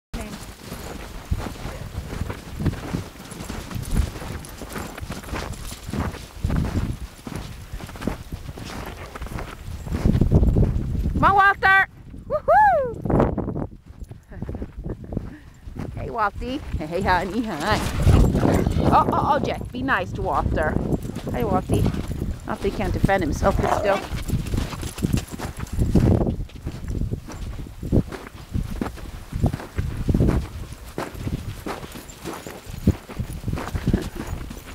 Ellie, Buckley, Kipper, Licorice, Jessie, Walter and Lucy having fun in snow
Fun in snow